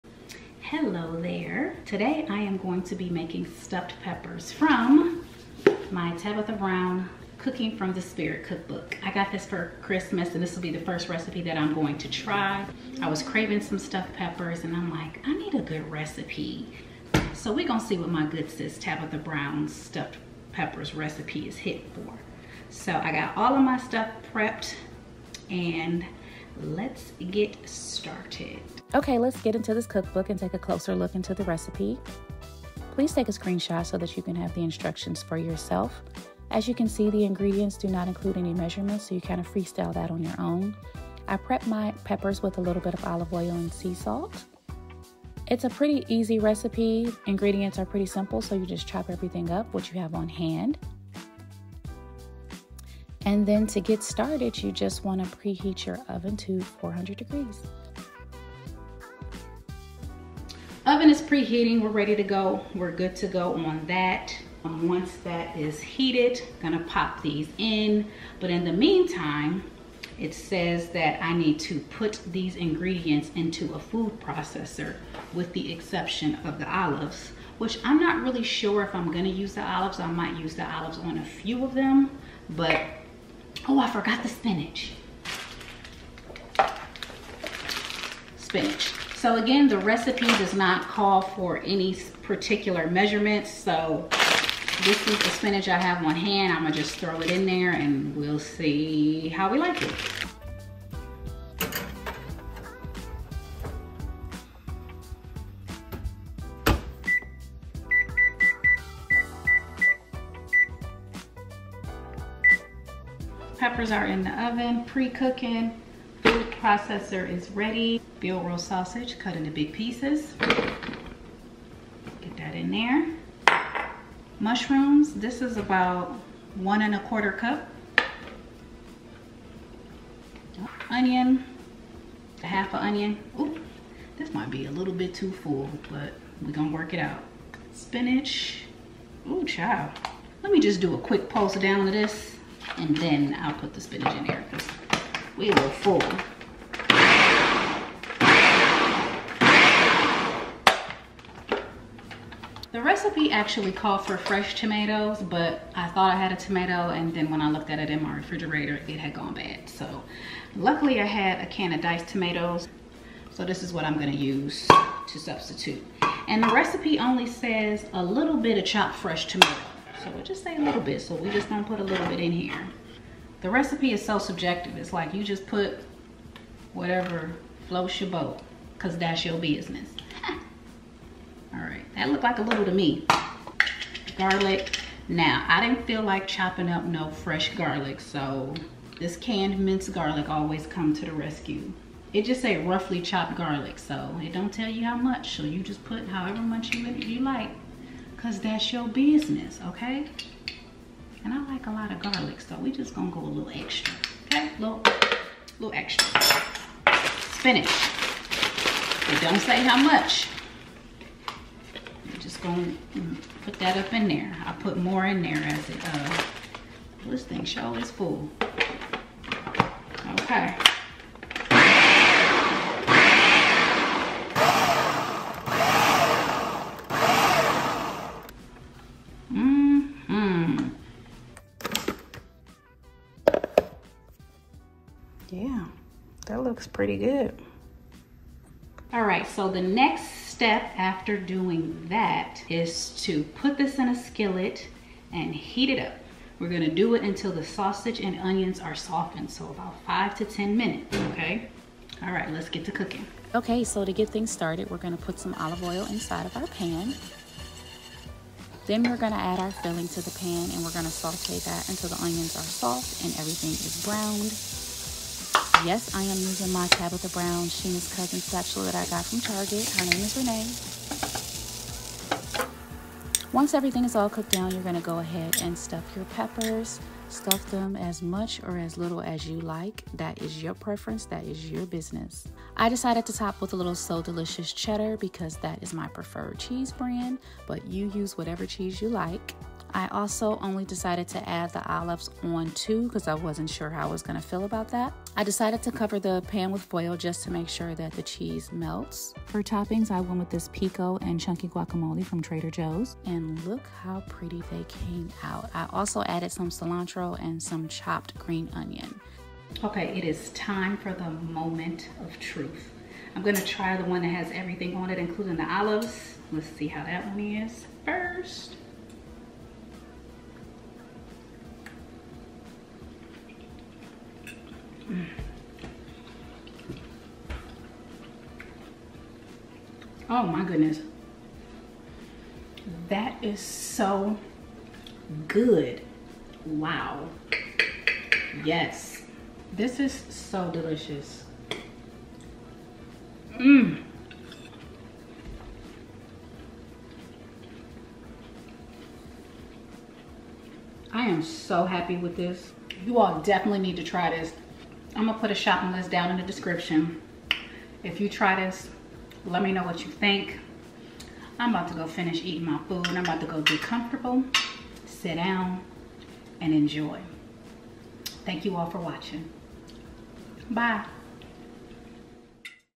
RECIPE REVIEW | TABITHA BROWN'S VEGAN STUFFED PEPPERS
I was so excited to try out a recipe from Tabitha Brown's cookbook Cooking From The Spirit. I was craving stuffed peppers and she had the perfect quick and easy recipe. Grab a screenshot of the recipe from the video but here are the ingredients.

Shopping List:
EVOO
Peppers (whichever color you prefer)
Vegan Sausage (used Field Roast Apple & Sage)
Mushrooms
Fresh Spinach
Tomato
Purple Onion
Garlic
Vegan cheddar cheese
Sliced black olives

Optional Toppings:
Avocado (used guacamole )
Pico de Gallo
Sea Salt
Black Pepper

*I forgot to use my microphone so the audio quality could be better. Cut me some slack*